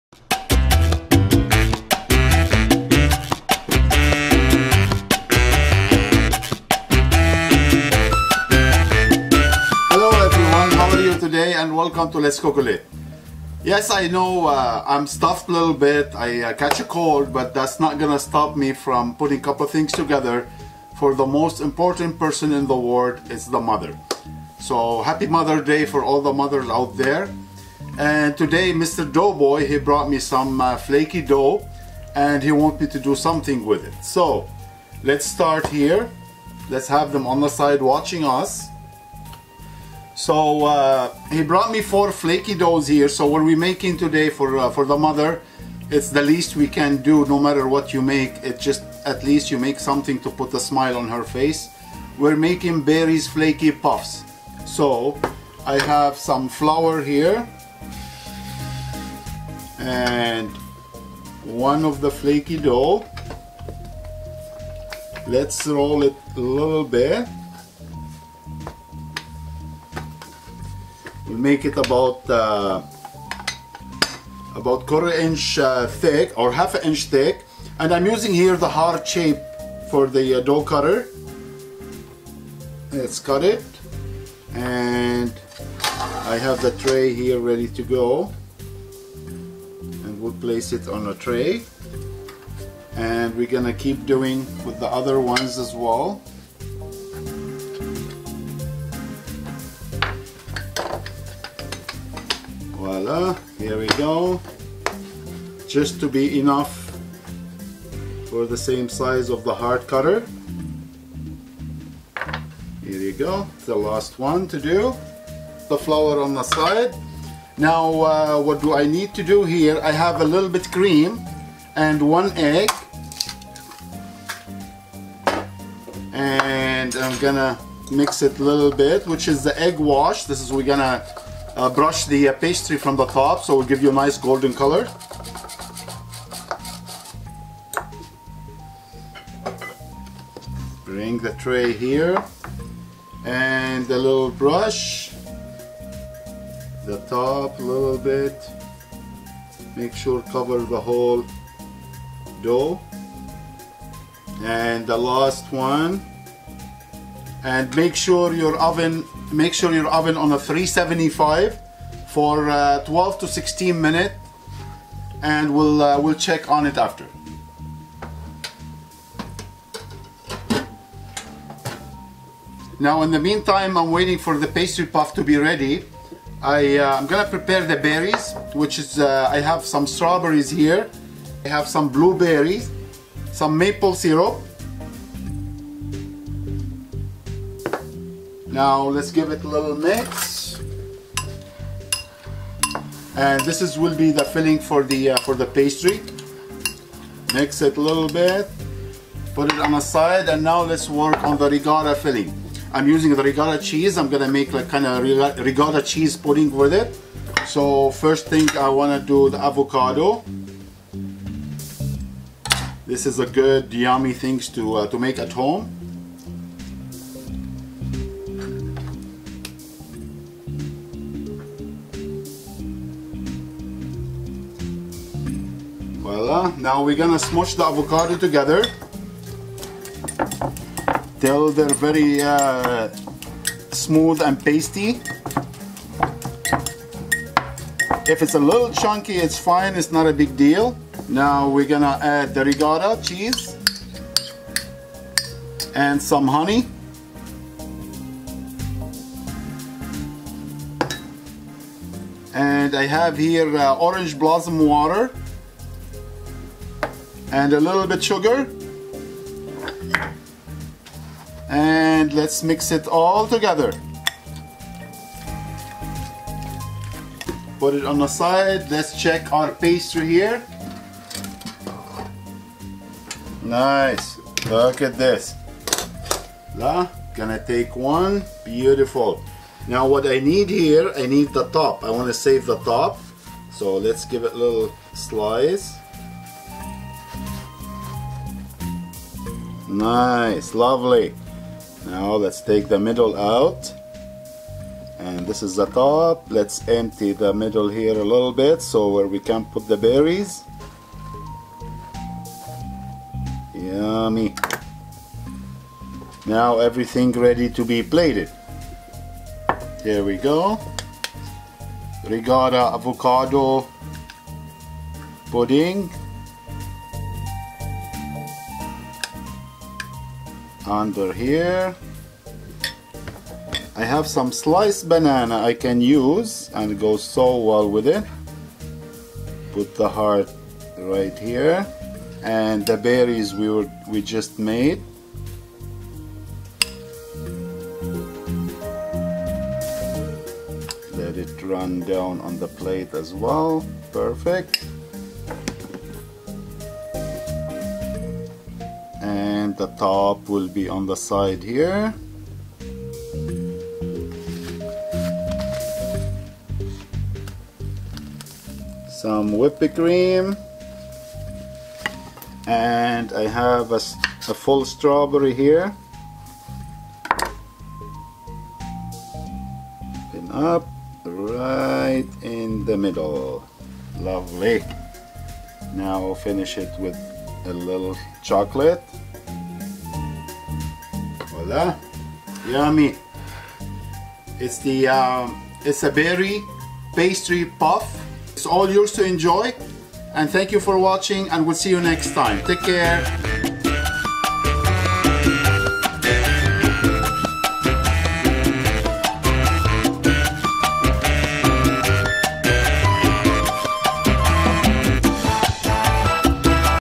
Berry Pastry Puff
Whenever you need a quick and delicious sweet treat no matter the occasion you might consider this Berry Puff Pastry with a homemade avocado ricotta cheese pudding. It looks great and taste yummy , All yours to ENJOY!!